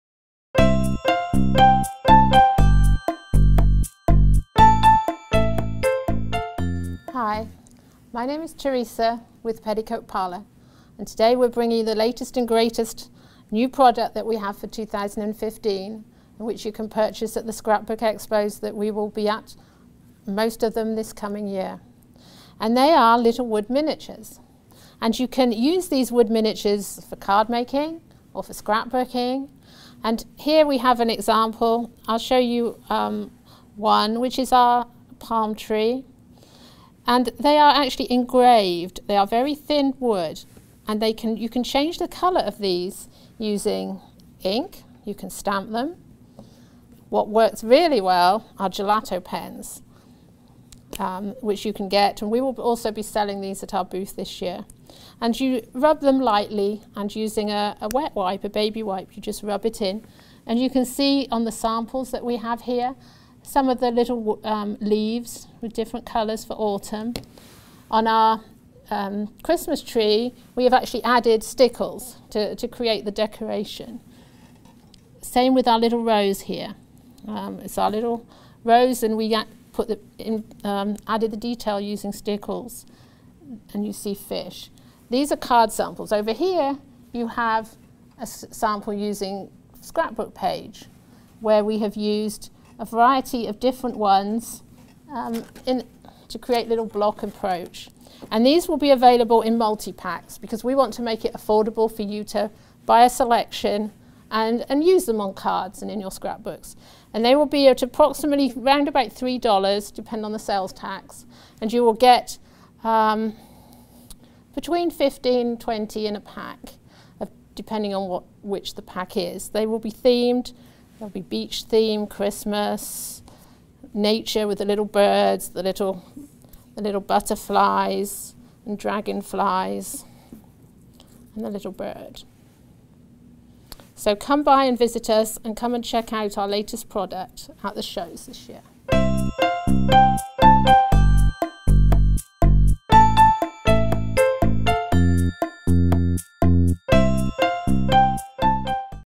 Latest & Greatest: Wood Miniatures from Petticoat Parlor
Teresa from Petticoat Parlor demonstrates their Wood Miniatures, created with beautifully engraved thin wood. You can easily color them with pens, stamps, etc.